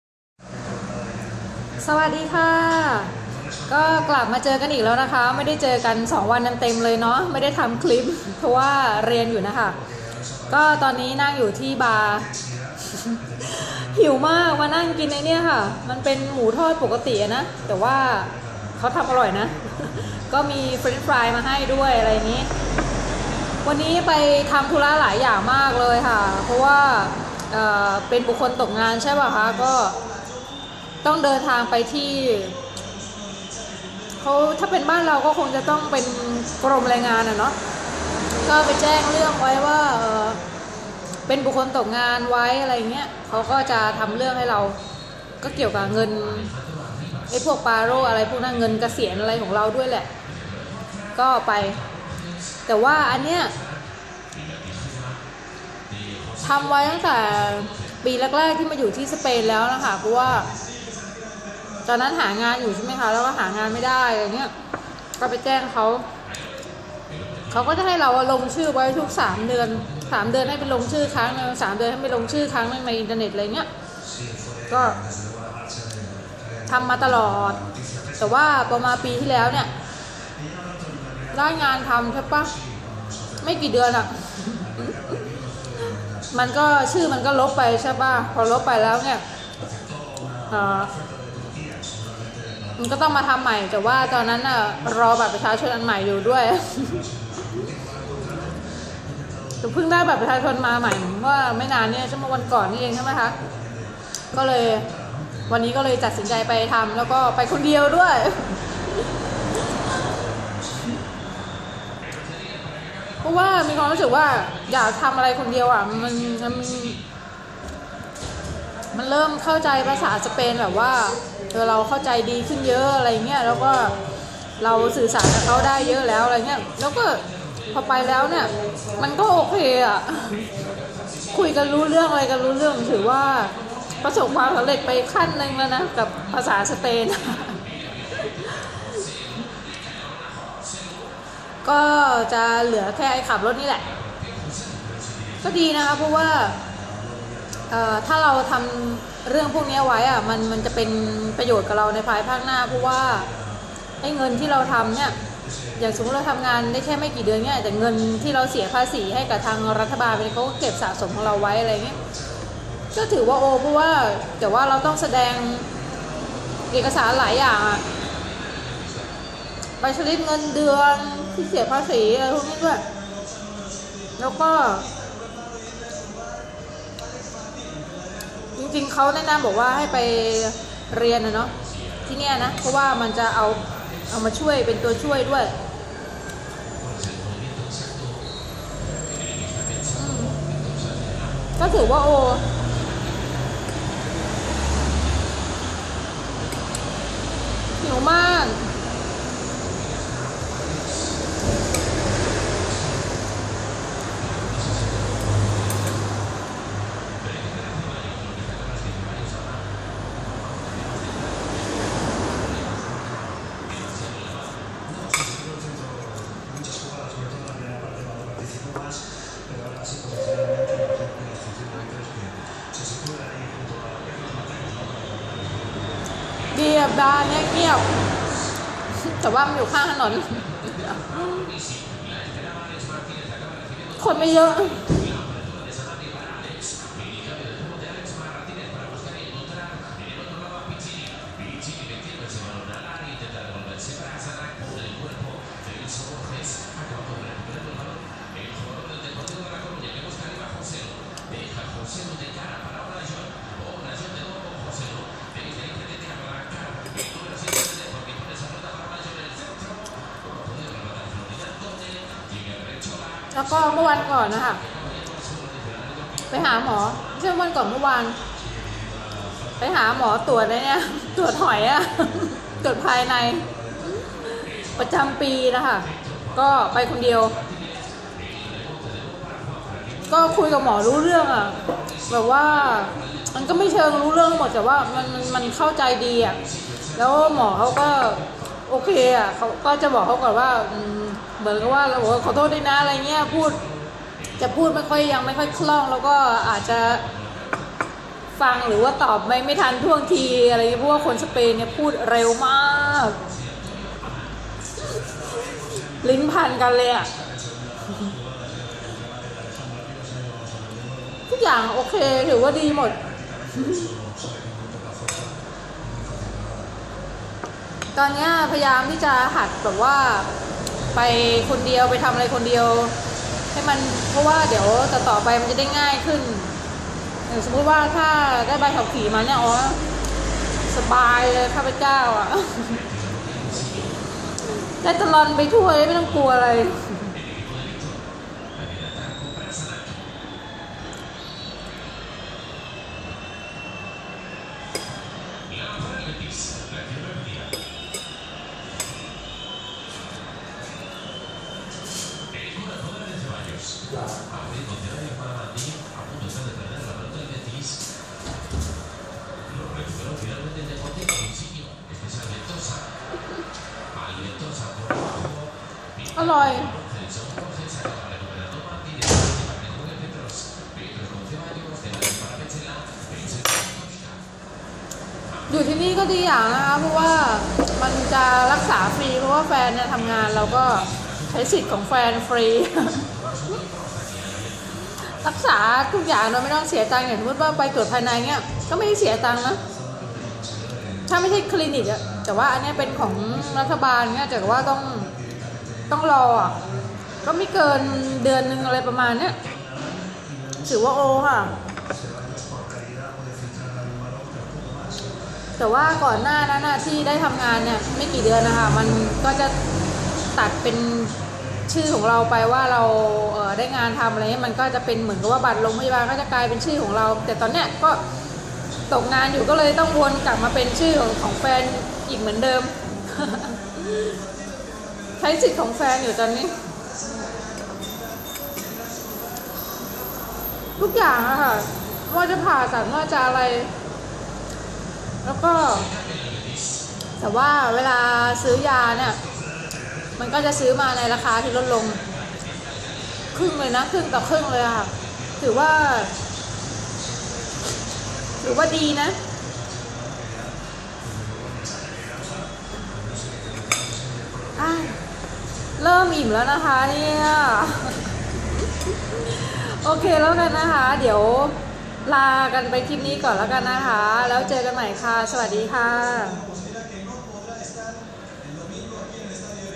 เม้าท์คนไม่มีงานทำ มาอยู่สเปนรีบไปที่กรมแรงงาน ไม่มีงานทำไม่ใช่เรื่องน่าอายค่ะ
ไม่มีงานทำไม่ใช่เรื่องน่าอายค่ะ ไม่จำเป็นต้องมานั่งโกหกใคร ๆ ด้วยว่ามีงานทำ  ให้ดูดีค่ะ เค้าตรวจสอบกันได้แบบง่าย ๆ รีบไปกรมแรงงานแจ้งเรื่องไว้จะได้ง่ายดายค่ะ มีงานที่ตรงตามความสามารถของเราเค้าจะได้เรียกเราทำงานค่ะ